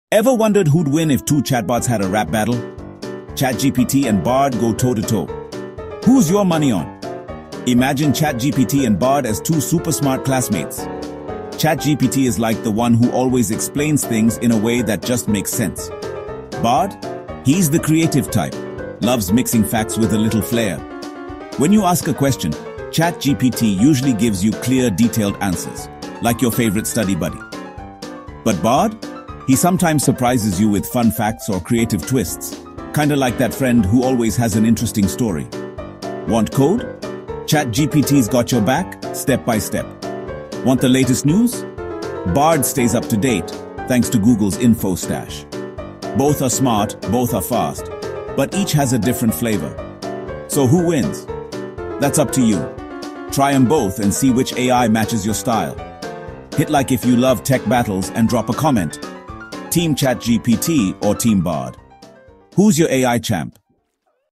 ChatGPT vs Bard: AI Showdown in 60 Seconds!
Which AI is smarter? Faster? More helpful? We put ChatGPT and Google Bard head-to-head to find out who wins in real-world tasks. 🧠⚔️